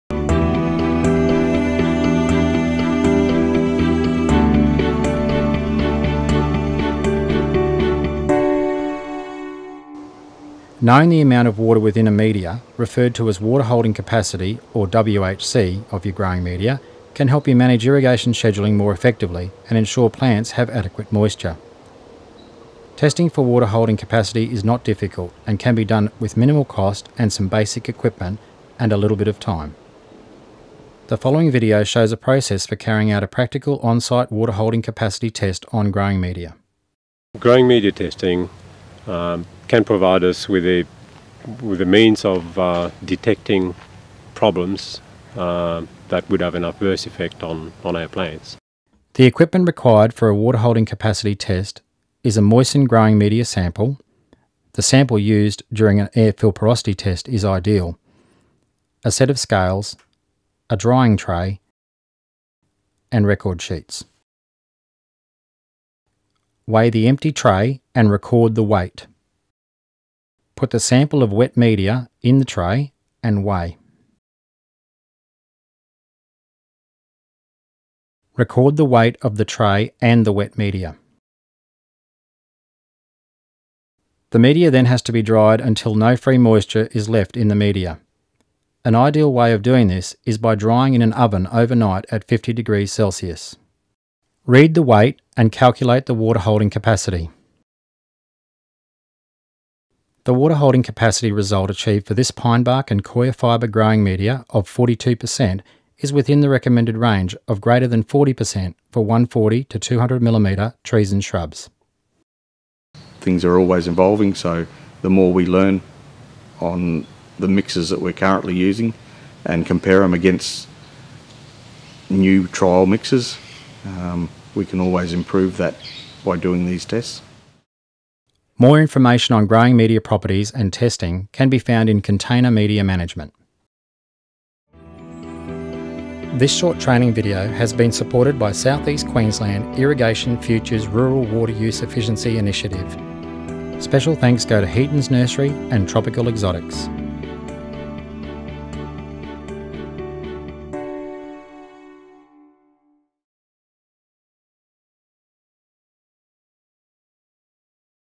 Growing Media Water Holding Capacity (WHC) Testing
Knowing the amount of water held within a growing media, referred to as  Water Holding Capacity (WHC) of your growing media can help you manage irrigation scheduling more effectively and ensure plants have adequate moisture. Testing for Water Holding Capacity isn't  difficult and can be done at minimal cost with some basic equipment and a little bit of time. This video shows a process for carrying out a practical on-site water holding capacity test on growing media.